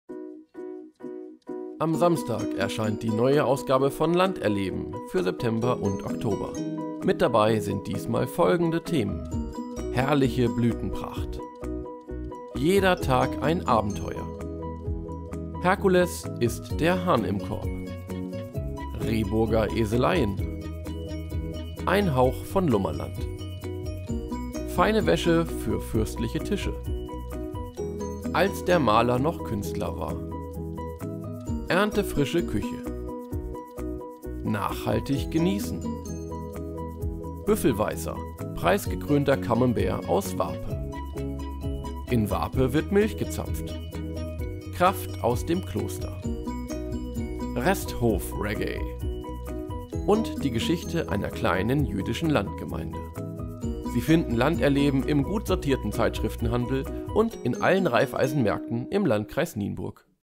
Land erleben Ausgabe 4/2015 - September/Oktober
Am 29. August 2015  erscheint die neue Ausgabe von Land erleben für September und Oktober. Mit dabei sind diesmal folgende Themen:

- Herrliche Blütenpracht
- Jeder Tag ein Abenteuer
- „Herkules“ ist der Hahn im Korb
- Rehburger Eseleien
- Ein Hauch von „Lummerland“
- Feine Wäsche für fürstliche Tische
- Als der Maler noch Künstler war
- Erntefrische Küche
- Nachhaltig genießen
- Büffelweißer: Preisgekrönter Camembert aus Warpe
- In Warpe wird Milch gezapft
- Kraft aus dem Kloster
- Resthof-Raggae
- Die Geschichte einer kleinen jüdischen Landgemeinde

Sie finden „Land erleben“ im gut sortierten Zeitschriftenhandel und in allen Raiffeisenmärkten im Landkreis Nienburg.